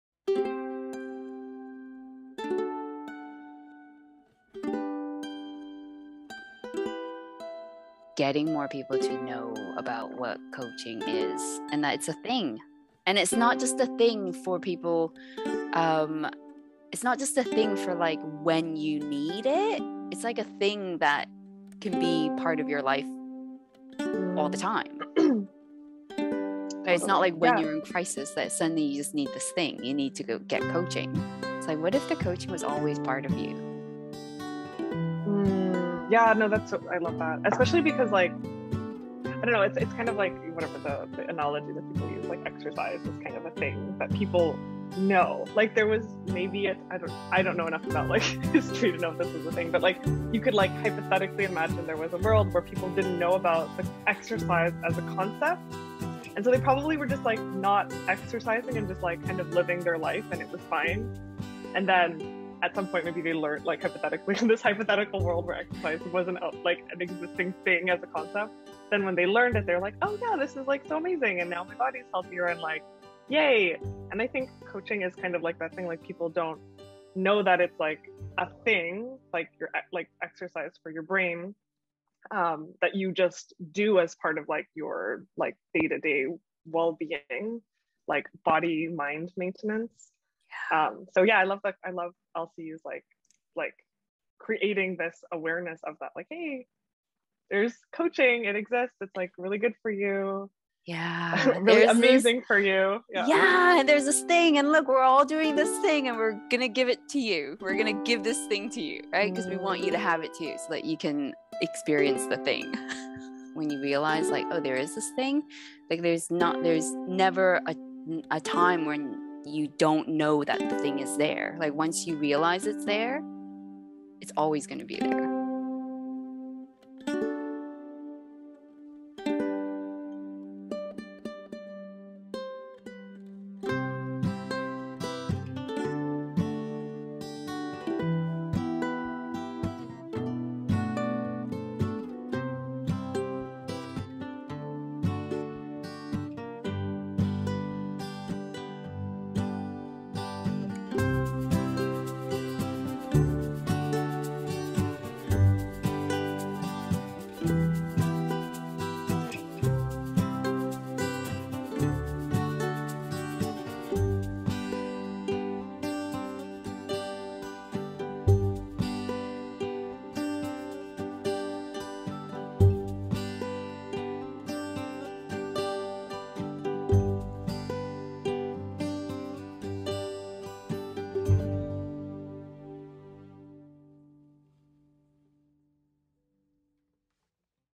Imagine a world where everyone has "The Thing"?
Alana Schramm, Joy and Intuition Coach and Dien Luu, Founder of Life Coach University talk about "The Thing".